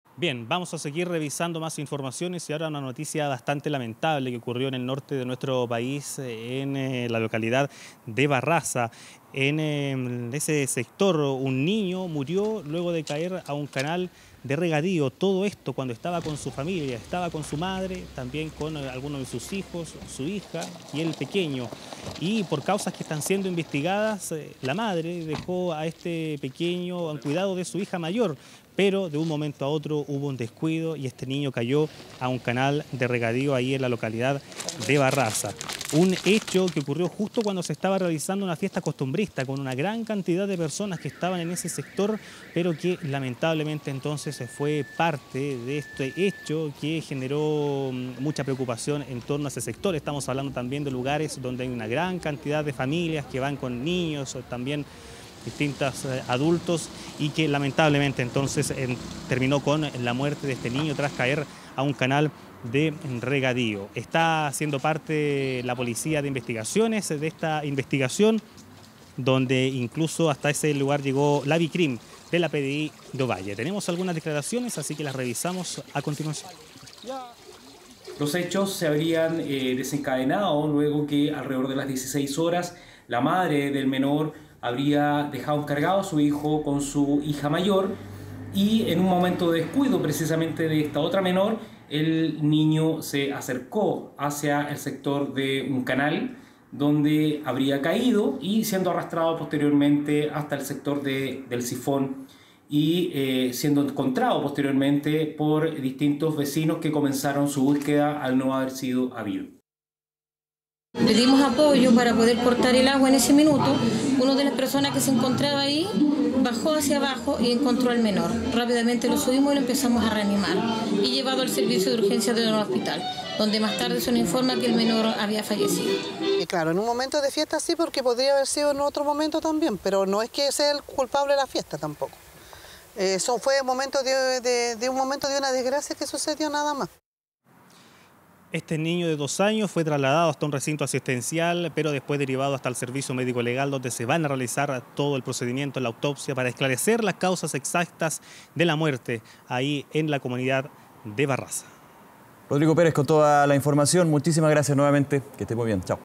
Niño de dos años falleció tras caer a canal de regadío en fiesta costumbrista en Ovalle
La familia habría perdido de vista al niño mientras instalaban una carpa, en la tarde de este sábado.